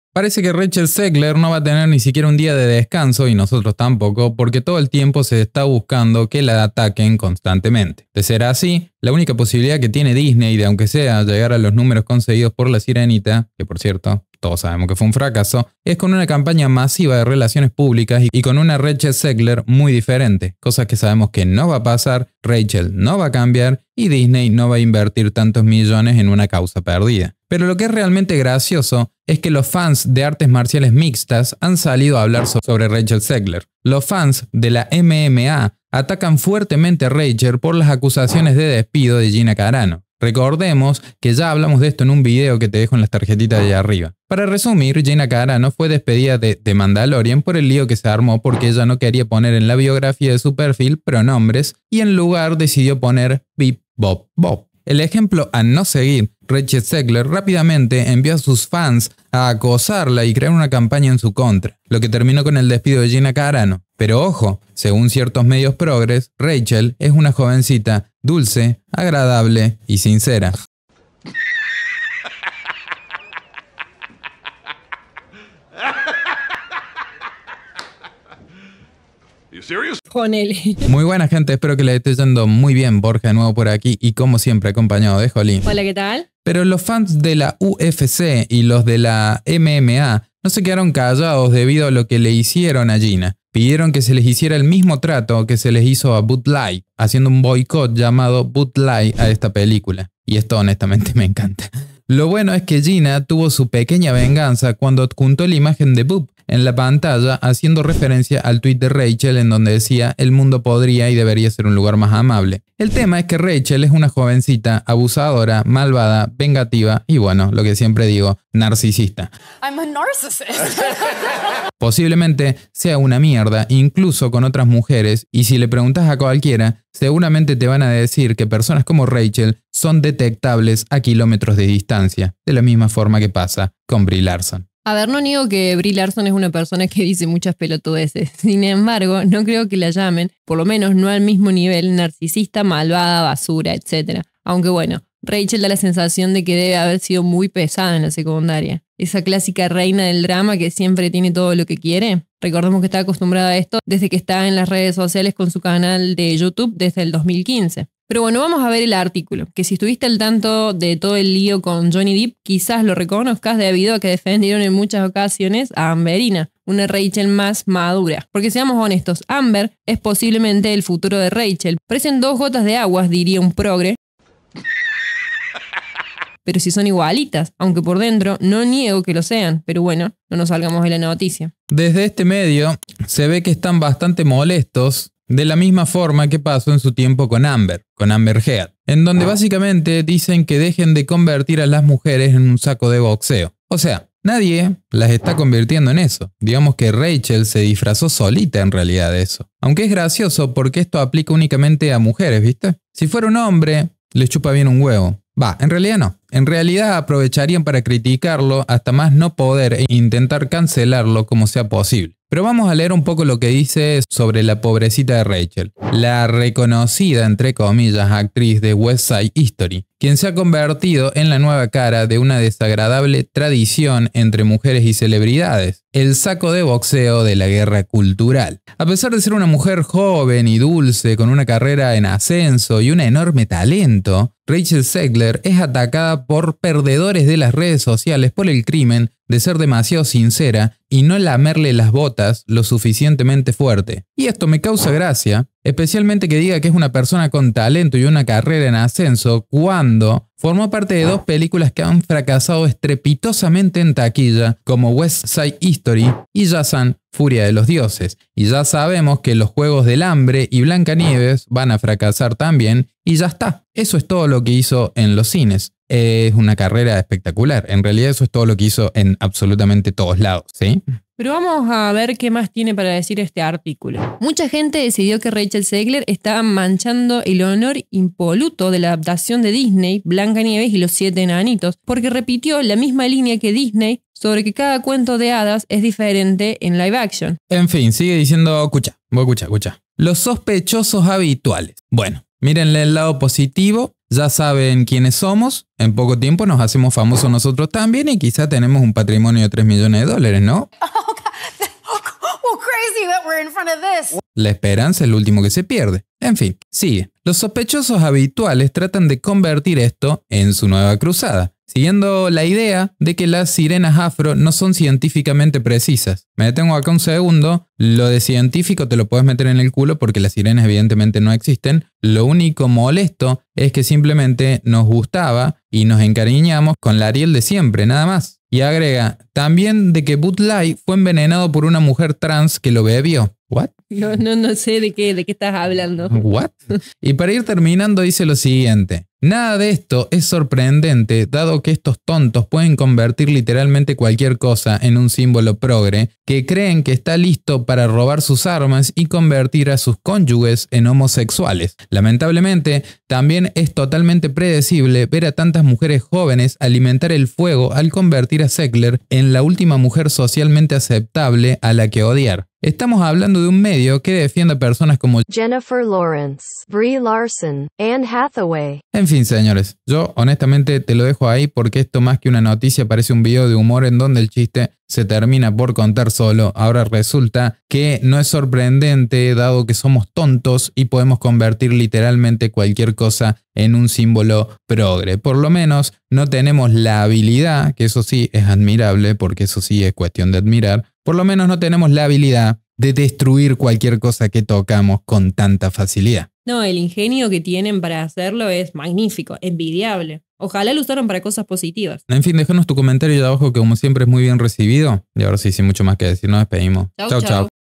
Rachel Zegler DULCE Y TALENTOSA 🤣 Medios PROGRES nos ATACAN por JUZGAR a BLANCANIEVES de DISNEY
Varios medios defienden a RachelZegler, pero es que no hay relato que pueda aguantar frente a todas las cosas que dice la próxima Blancanieves de Disney que no puede lograr que se calle de una buena vez.